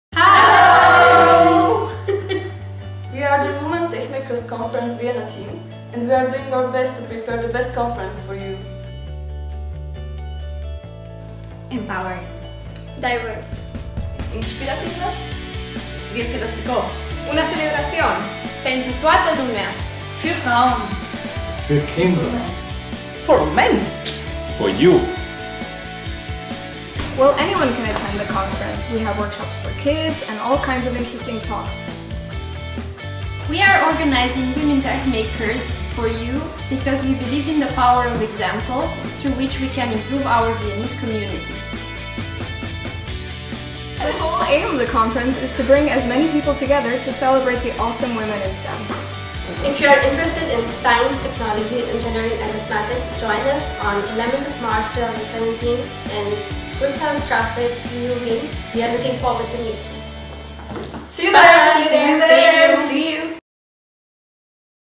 Women Techmakers Vienna 2017 invitation!
WTM Vienna 2017 Team invites you to join them on March 11, 2017.